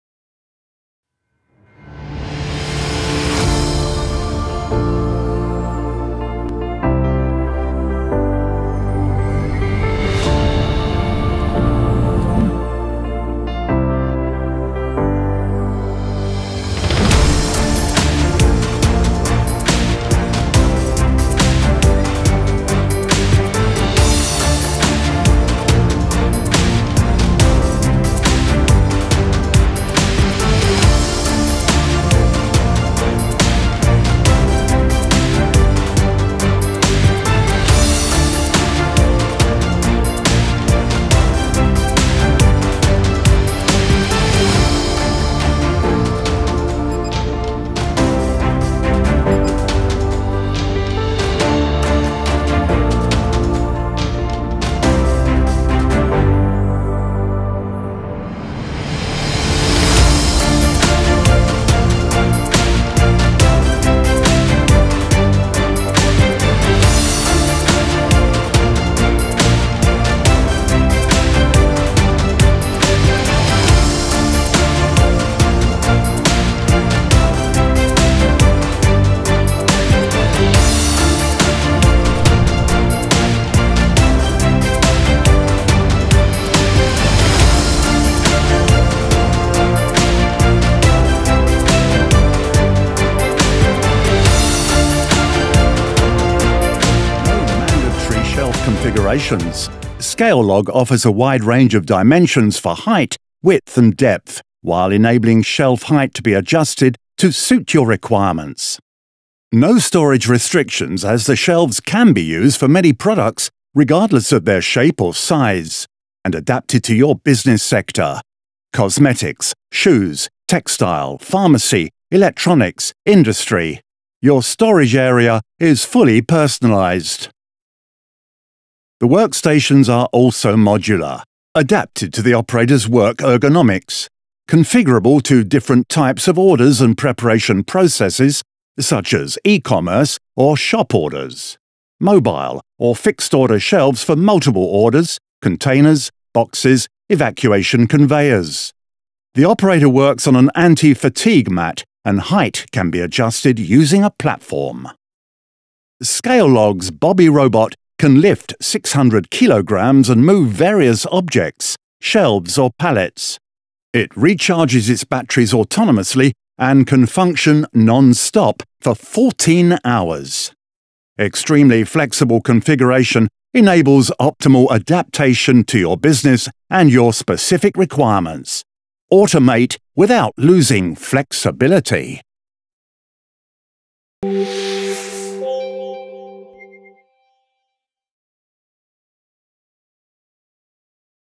Scallog-FTS "Goods-to-Person"
Verschiedene Hersteller wie Scallog, Grey Orange, Magazino, ICS und andere wetteifern um den Markt der Klein-FTS, welche Regale proaktiv zum Kommissionierer bringen. Derartige System werden in Logistikzentren eingesetzt, sie sollen sich auch für die Produktionsversorgung eignen. Hier die Systembeschreibung des französischen Systems. Mehr dazu in FTS-AGV-FACTS 01-2020 zur LOGIMAT